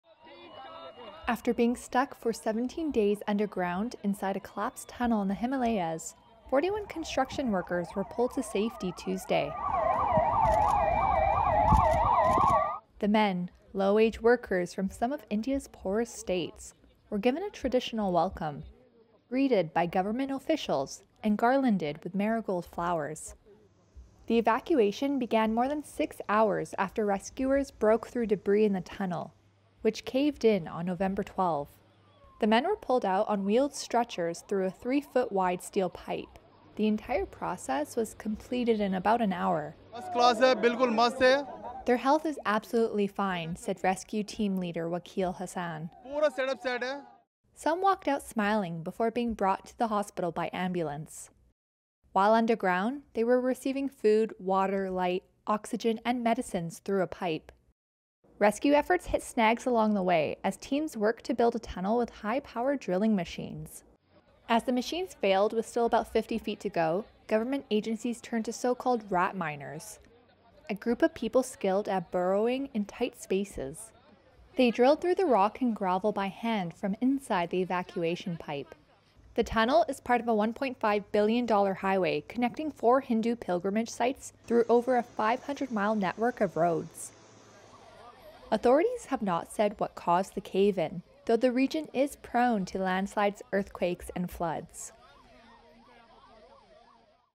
Trapped Indian workers freed after 17 days
After being trapped underground for 17 days in a collapsed tunnel in the Himalayas, 41 construction workers were rescued. They were pulled to safety after rescue workers drilled through the debris of rock, concrete, and earth to reach them.